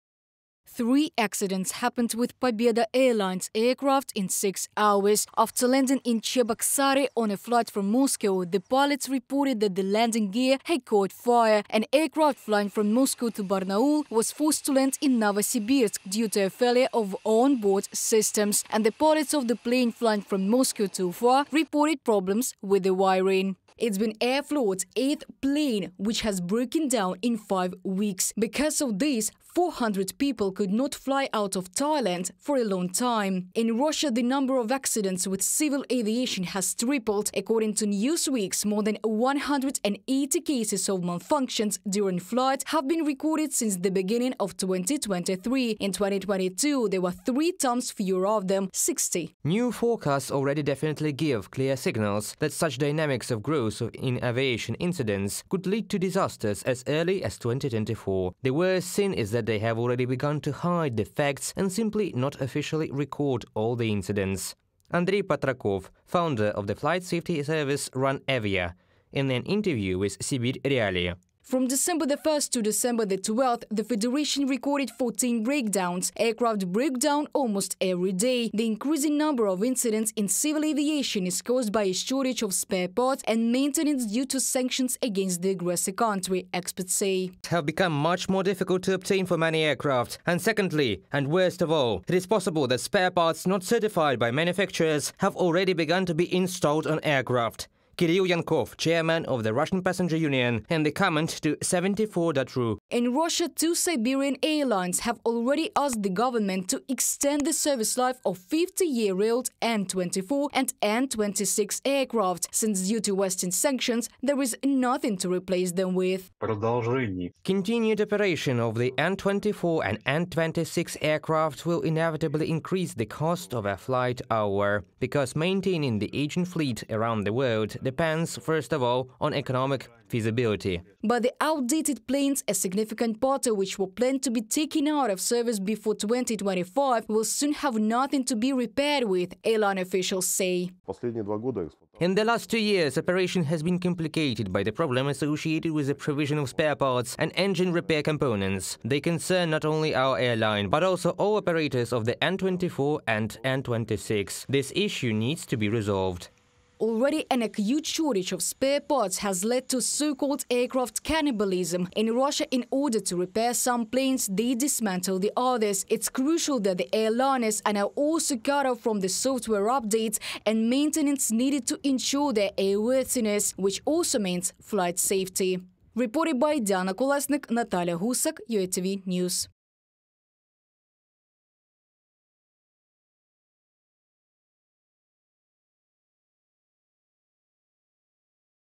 Civilian Airlines Are DANGEROUS in Russia: Die if You Fly
Spare parts for Western and Ukrainian-made planes in Russia are in short supply, to put it mildly. Because of sanctions and the 'nationalisation' of leased Boing and Airbus passenger planes, Russian airlines can no longer use them safely. No maintenance, no spare parts.